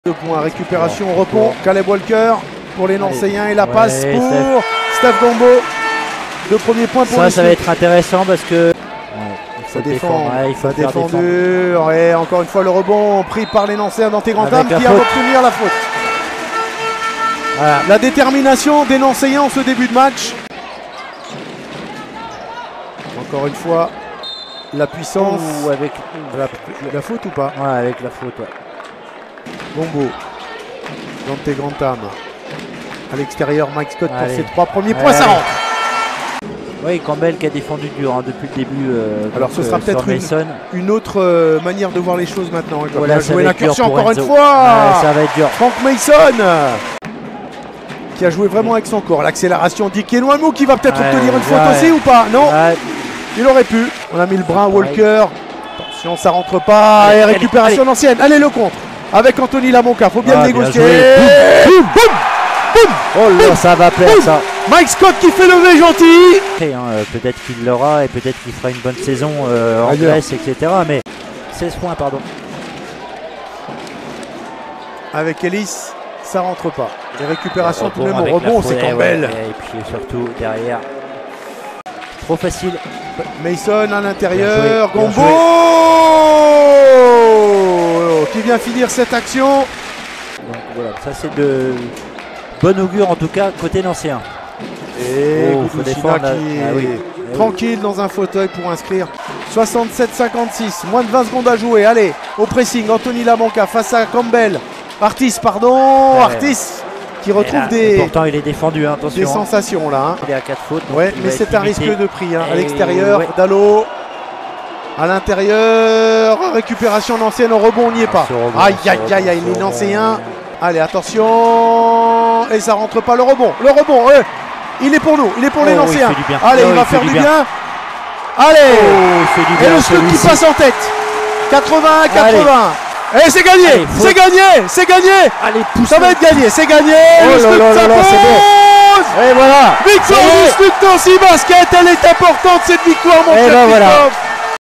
Résumé : SLUC Nancy Basket vs Cholet Basket (19ème Journée Betclic Elite)
Résumé complet de la rencontre entre le SLUC Nancy Basket et le Cholet Basket !